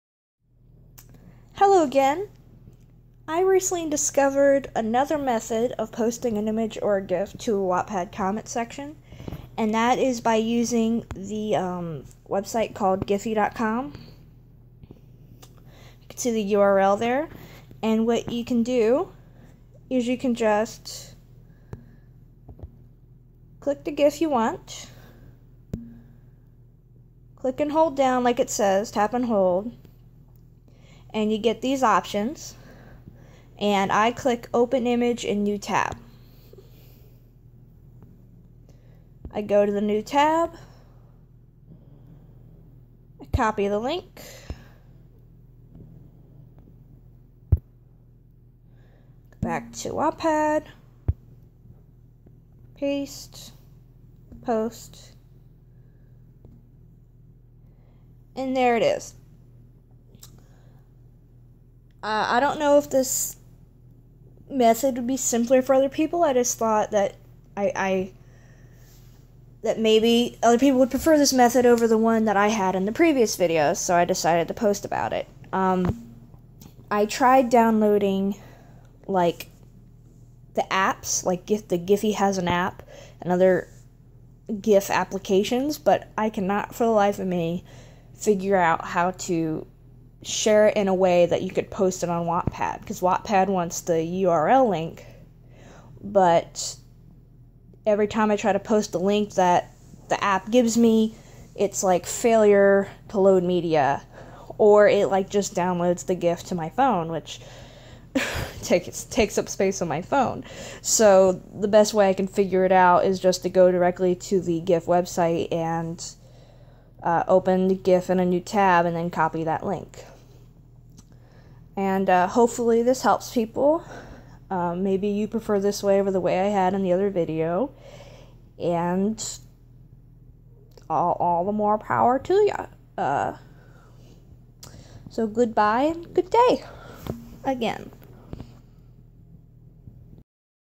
Wattpad Mobile: How to Comment an Image or Gif using Giphy (HAS ISSUES)
Just another method of posting gifs to Wattpad comment sections via giphy.com. Giphy went weird and hates me, so I made a short troubleshooting video THERE IS ALSO A BETTER METHOD VIA TENOR